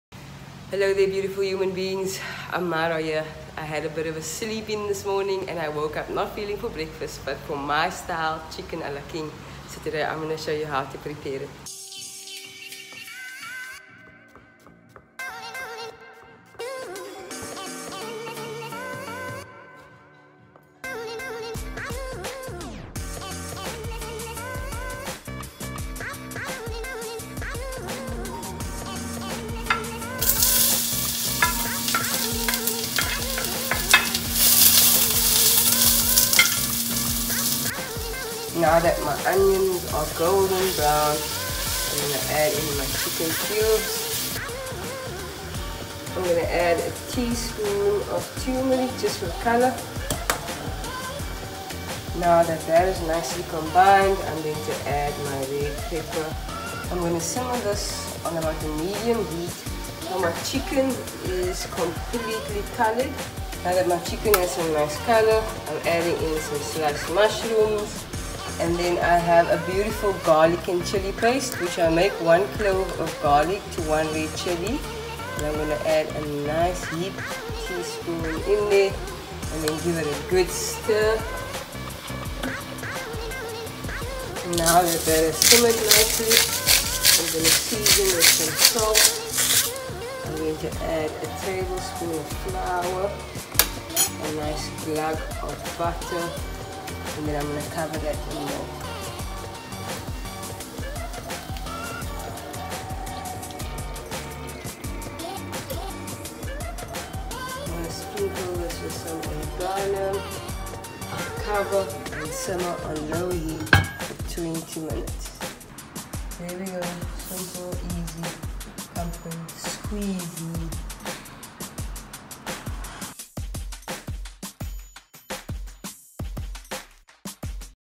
How to make Chicken Ala King
How to make chickcreamy chicken and mushroom casserole. A simple, quick meal to share and prepare.